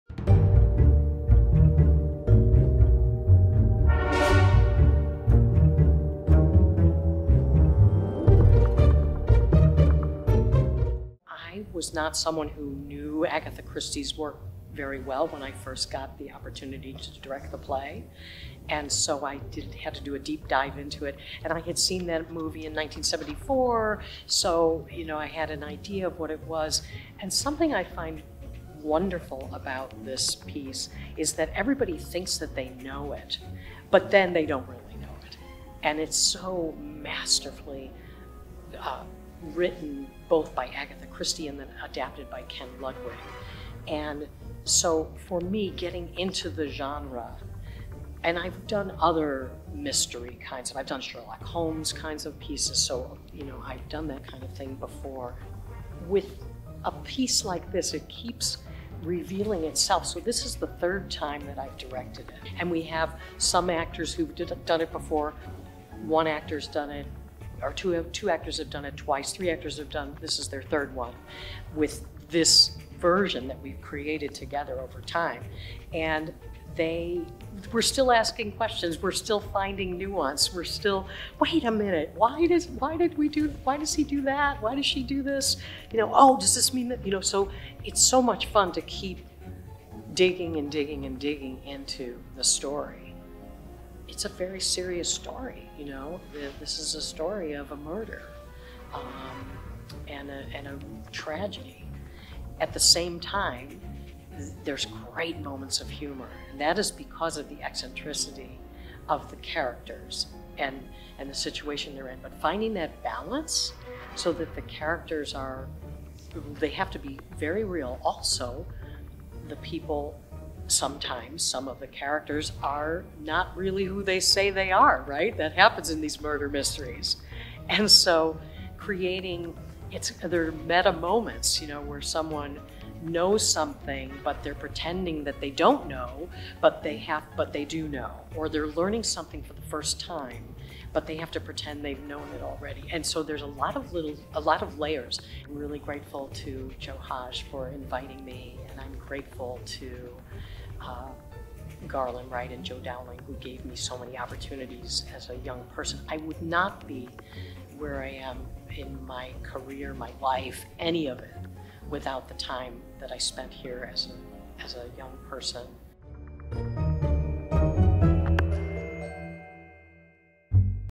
Stage Craft with Risa Brainin
Hear from MURDER ON THE ORIENT EXPRESS Director Risa Brainin on what goes into bringing this who-done-it mystery to life on stage at the Guthrie Theater.

MURDER ON THE ORIENT EXPRESS
by AGATHA CHRISTIE
adapted for the stage by KEN LUDWIG
directed by RISA BRAININ
May 13 - July 2